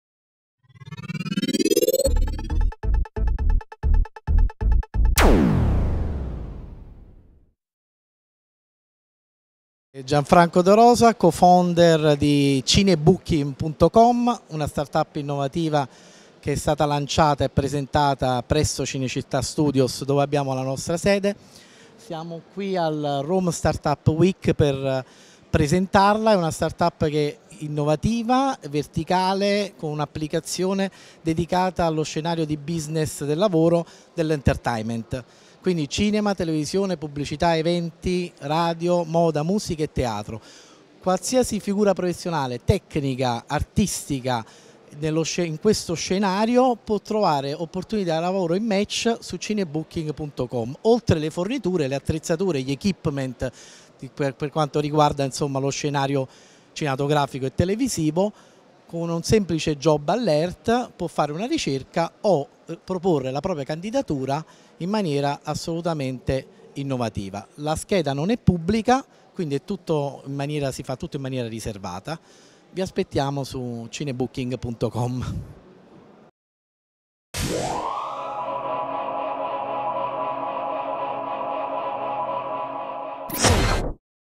Rome Startup Week, Intervista a Gianfranco De Rosa
Al Rome Startup Week, Pc-Magazine ha intervistato Gianfranco De Rosa, Cofounder di Cinebooking.com